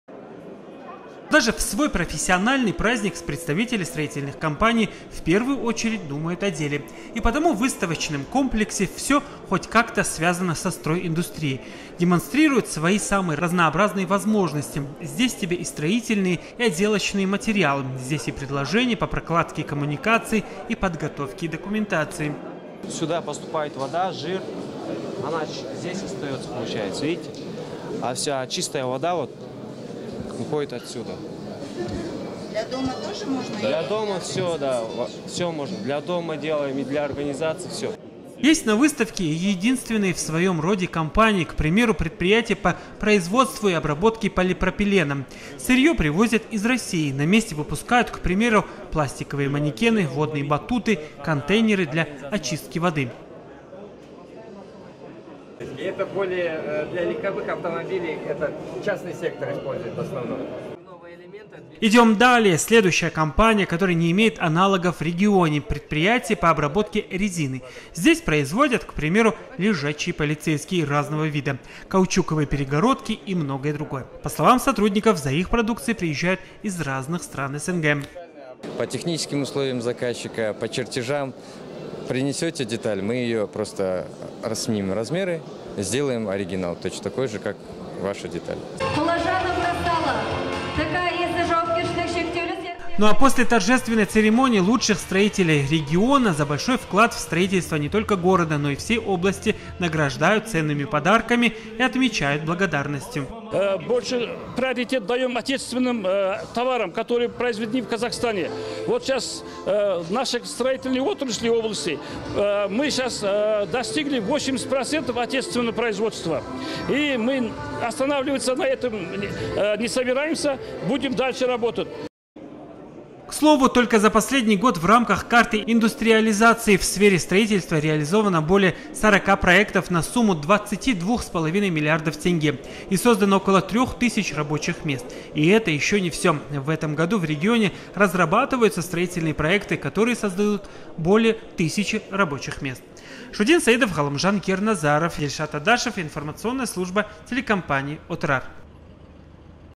Выставка стройндустрии ЮКО прошла в день строителя
В Шымкенте поздравляют всех работников стройиндустрии. Сегодня в выставочном комплексе прошла выставка продукции строительных компаний, где более 50 предприятий продемонстрировали весь спектр возможных услуг и товаров.